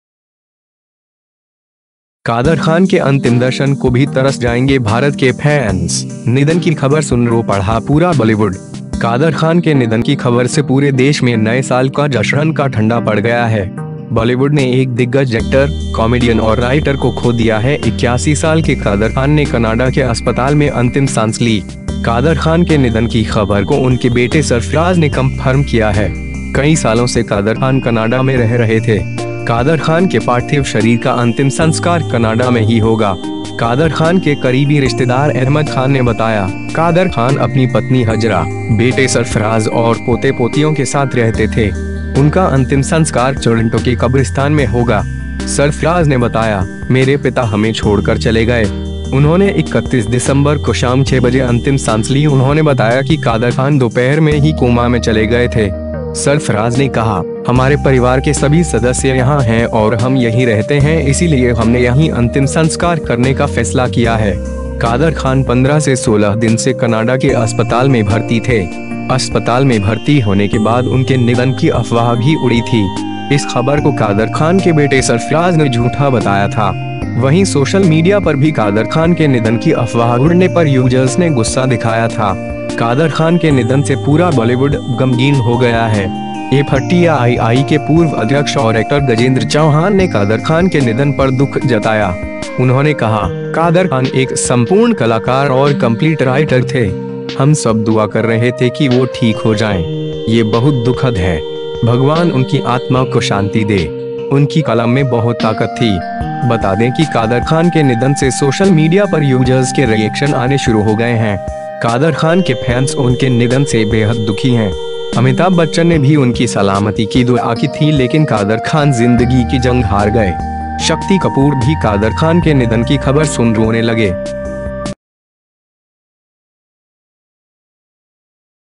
कादर खान के अंतिम दर्शन को भी तरस जाएंगे भारत के फैंस, निधन की खबर सुन रो पड़ा पूरा बॉलीवुड
कादर खान के निधन की खबर से पूरे देश में नए साल का जश्न का ठंडा पड़ गया है । बॉलीवुड ने एक दिग्गज एक्टर, कॉमेडियन और राइटर को खो दिया है । 81 साल के कादर खान ने कनाडा के अस्पताल में अंतिम सांस ली । कादर खान के निधन की खबर को उनके बेटे सरफराज ने कंफर्म किया है ।

कई सालों से कादर खान कनाडा में रह रहे थे । कादर खान के पार्थिव शरीर का अंतिम संस्कार कनाडा में ही होगा । कादर खान के करीबी रिश्तेदार अहमद खान ने बताया, 'कादर खान अपनी पत्नी हजरा, बेटे सरफराज और पोते-पोतियों के साथ रहते थे । उनका अंतिम संस्कार टोरंटो के कब्रिस्तान में होगा ।'

सरफराज ने बताया, 'मेरे पिता हमें छोड़कर चले गए। उन्होंने 31 दिसंबर को शाम 6 बजे अंतिम सांस ली।' उन्होंने बताया कि कादर खान दोपहर में ही कोमा में चले गए थे। सरफराज ने कहा, 'हमारे परिवार के सभी सदस्य यहां हैं और हम यहीं रहते हैं इसीलिए हमने यहीं अंतिम संस्कार करने का फैसला किया है।'

कादर खान 15-16 दिन से कनाडा के अस्पताल में भर्ती थे । अस्पताल में भर्ती होने के बाद उनके निधन की अफवाह भी उड़ी थी । इस खबर को कादर खान के बेटे सरफराज ने झूठा बताया था । वहीं सोशल मीडिया पर भी कादर खान के निधन की अफवाह उड़ने पर यूजर्स ने गुस्सा दिखाया था ।

कादर खान के निधन से पूरा बॉलीवुड गमगीन हो गया है । एफटीआईआई के पूर्व अध्यक्ष और एक्टर गजेंद्र चौहान ने कादर खान के निधन पर दुख जताया । उन्होंने कहा, 'कादर खान एक संपूर्ण कलाकार और कंप्लीट राइटर थे । हम सब दुआ कर रहे थे कि वो ठीक हो जाएं । ये बहुत दुखद है । भगवान उनकी आत्मा को शांति दे । उनकी कलम में बहुत ताकत थी ।'

बता दें कि कादर खान के निधन से सोशल मीडिया पर यूजर्स के रिएक्शन आने शुरू हो गए हैं । कादर खान के फैंस उनके निधन से बेहद दुखी हैं । अमिताभ बच्चन ने भी उनकी सलामती की दुअा की थी लेकिन कादर खान जिंदगी की जंग हार गए । शक्ति कपूर भी कादर खान के निधन की खबर सुन रोने लगे।